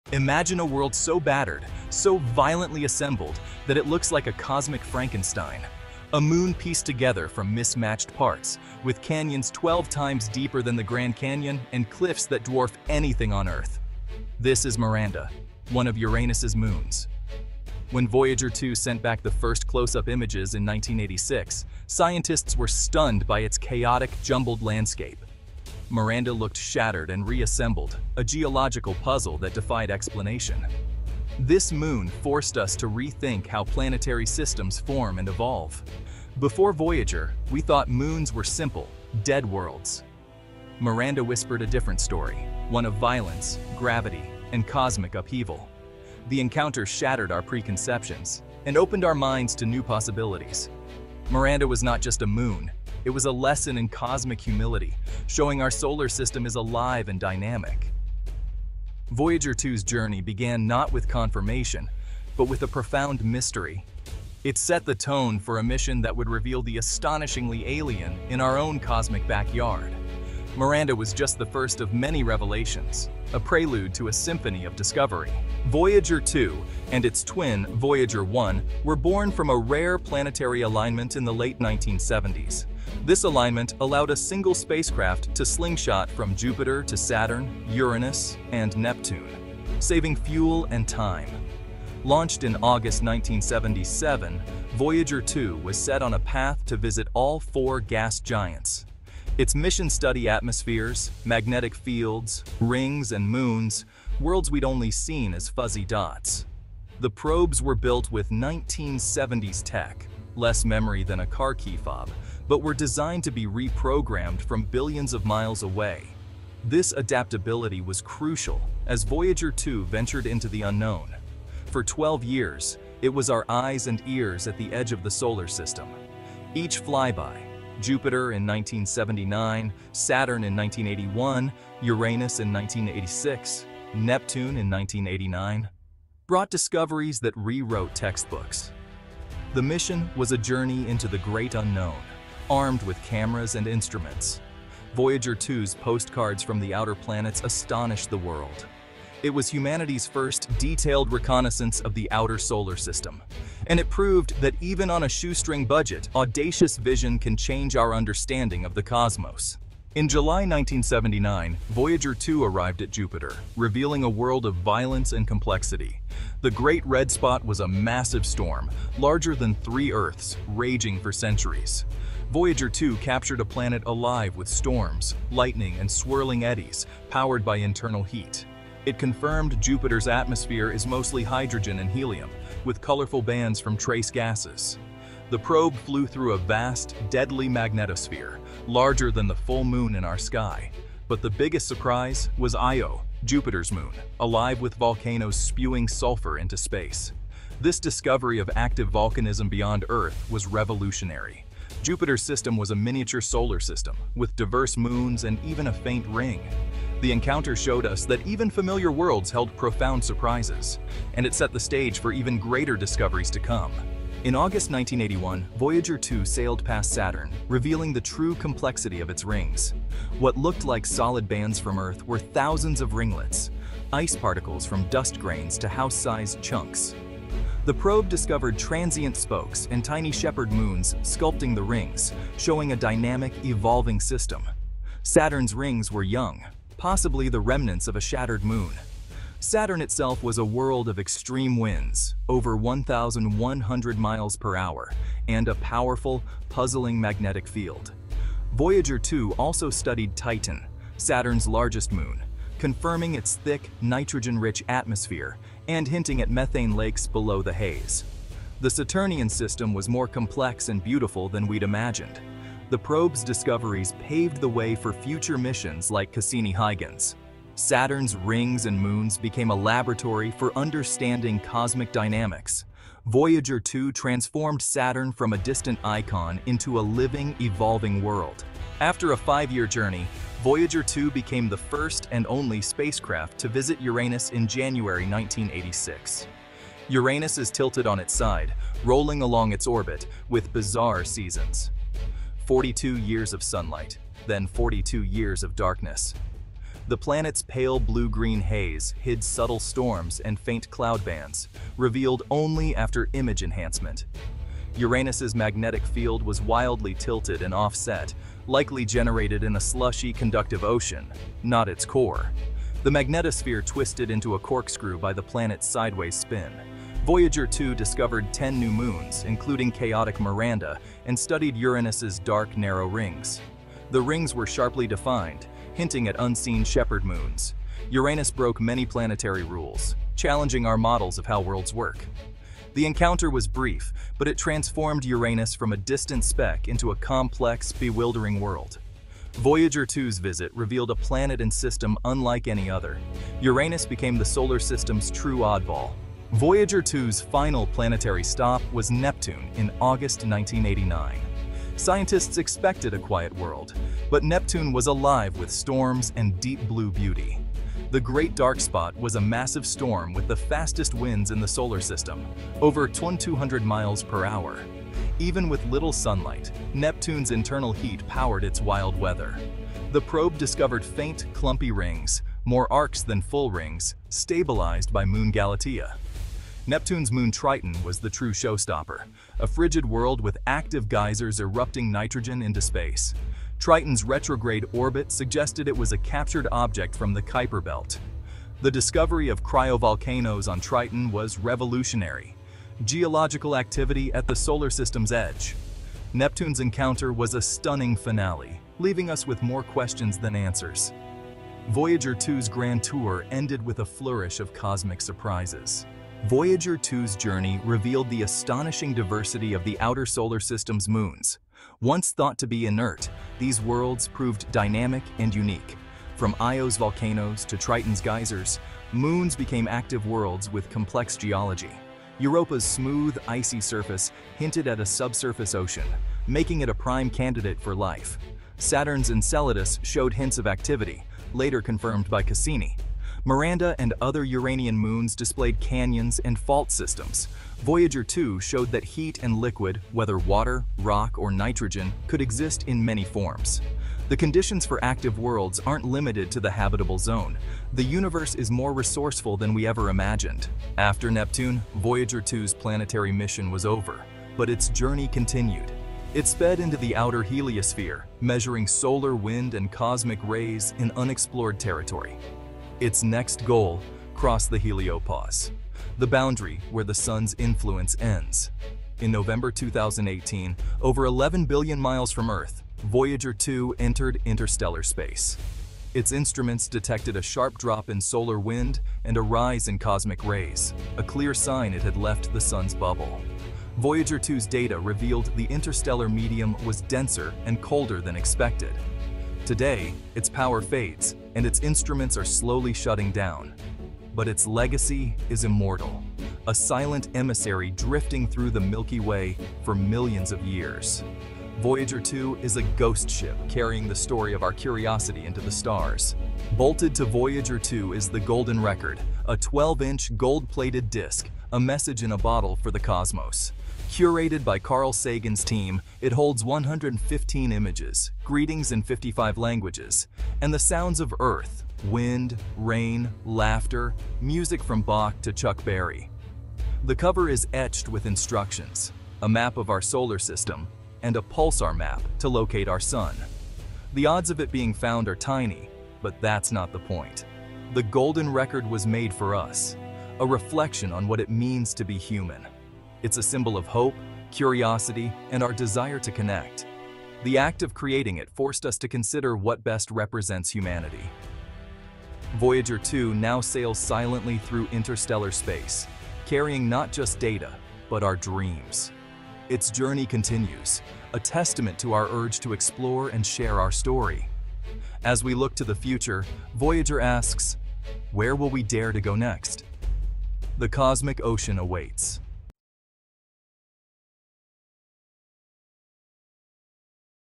Voyager 2 Discovers the Unexpected — A Turning Point in Humanity’s View of the Cosmos
Embark on an epic journey through the outer solar system with Voyager Two! Discover Miranda’s chaotic and mysterious terrain, unravel how Voyager’s 1986 images forever changed our understanding of moons, and witness the dramatic shift from seeing these worlds as lifeless to dynamic, geologically active entities. Explore Jupiter’s Great Red Spot, Io’s volcanic eruptions, Saturn’s intricate rings, Titan’s thick atmosphere, Uranus’s extreme seasons, and Neptune’s surprising storms. Learn how Voyager Two uncovered complex features like shepherd moons, transient spokes, and Triton’s geysers. As Voyager crosses into interstellar space, its Golden Record continues to symbolize humanity’s hope and curiosity.

OUTLINE:

00:00:00 The Whispers of Miranda
00:01:15 A Grand Tour on a Shoestring Budget
00:02:39 A World of Furious Storms
00:03:34 The Lord of the Rings Reveals Its Secrets
00:04:38 The Tilted Enigma
00:05:46 The Great Dark Spot and Triton's Cryovolcanoes
00:06:46 The Unexpected Diversity of Moons
00:07:30 Into Interstellar Space
00:08:26 A Message in a Bottle